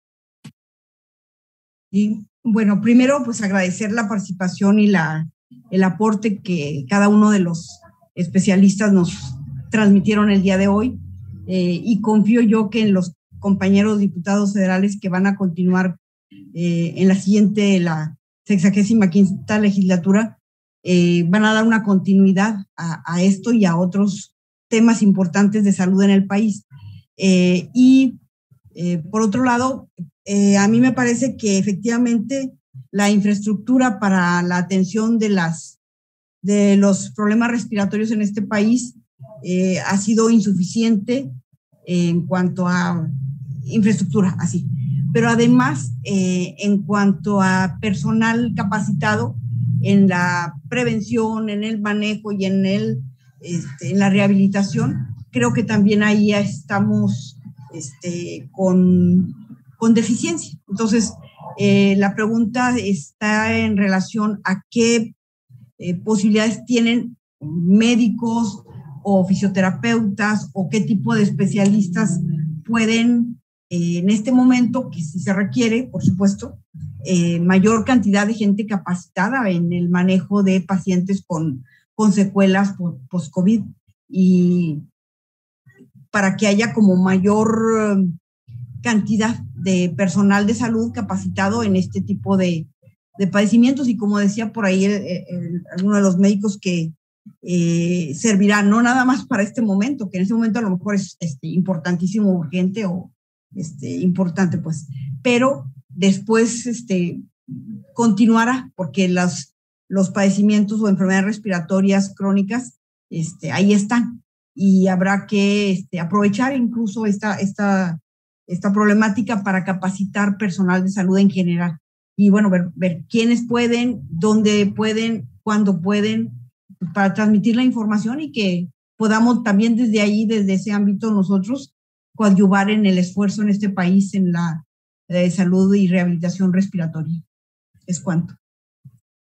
10/08/2021 Segunda participación de la Dip. Miroslava Sánchez Galván
Segunda participación de la Dip. Miroslava Sánchez Galván durante el Foro: Tratamientos respiratorios para la atención de pacientes post Covid-19 y secuelas.